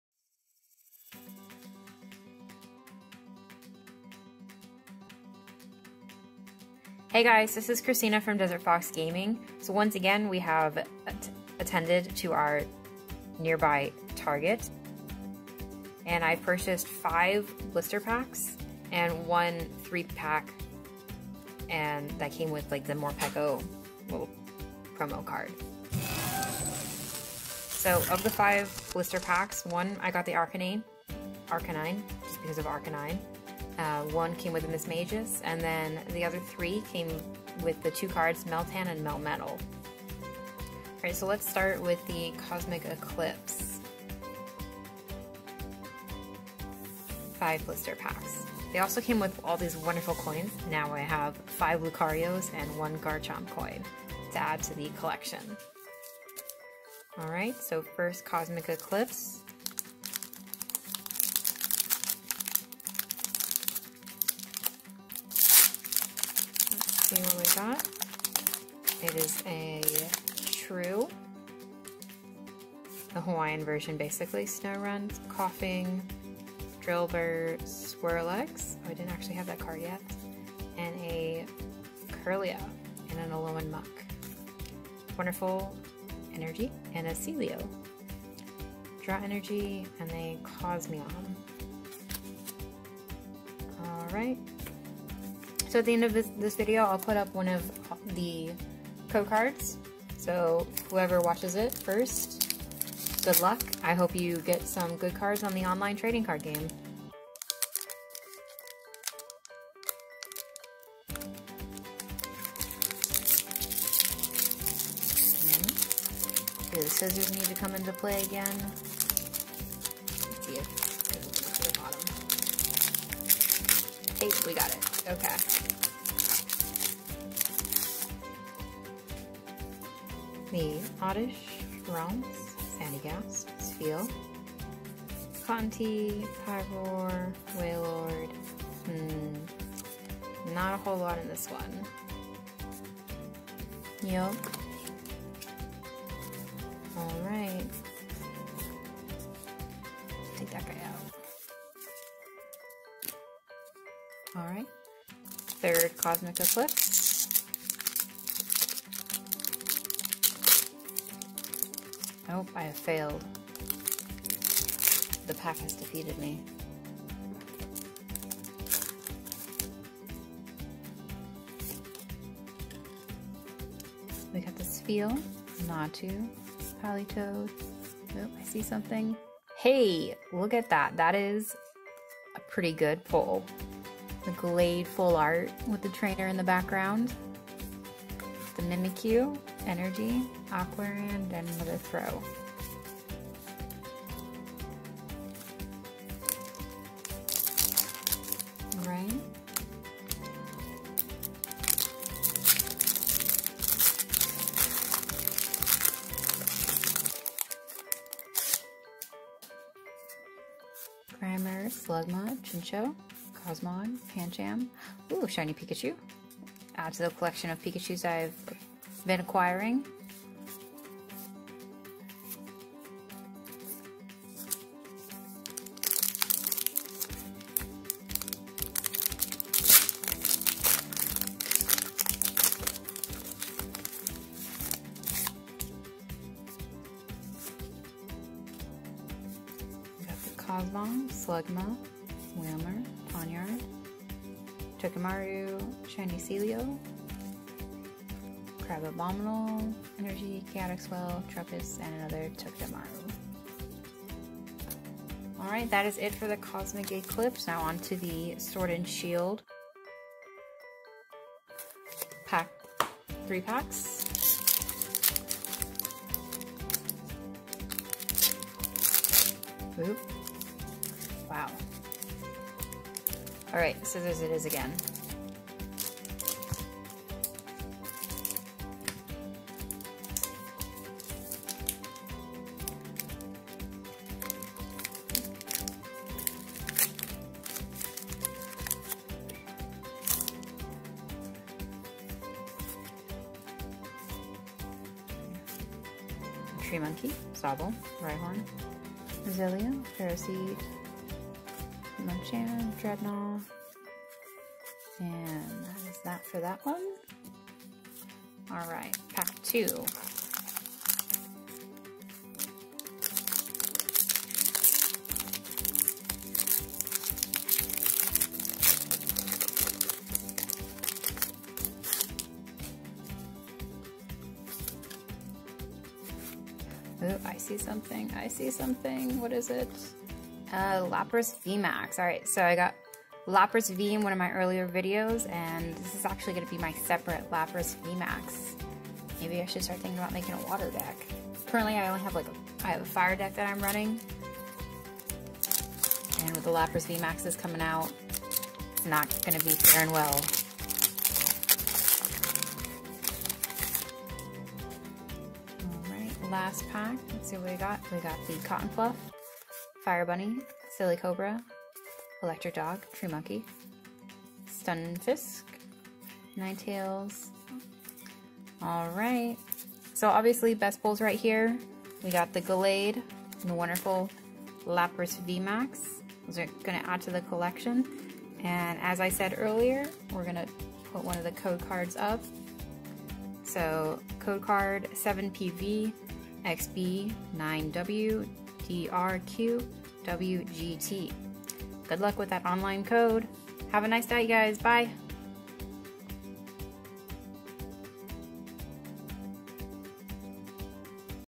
Blister Pack Opening
Five Blister Packs, with the top cards of Arcanine, Mismagius, Meltan and Melmetal (x3). One Morpeko promo three pack Sword and Shield. Code card for online TCG at the end!